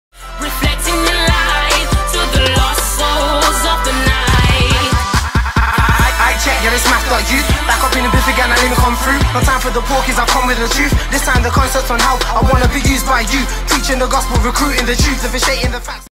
Support @mafdotyou at Affinity 3 15.11.14 @GL360 @the_drum
GL360’s Affinity 3

A night not to be missed featuring some of the best UK talent with 17 artists, a full live band, including the likes of Faith Child, Natty Joshia, Naomi ‘Namz’ Elliot , David B, Martha J & many others.

Sat Nov 15th at The Drum, Potters Lane, Aston, Birmingham
Tickets are £7.50, £5 Concessions

For more info follow @GL360 on twitter & search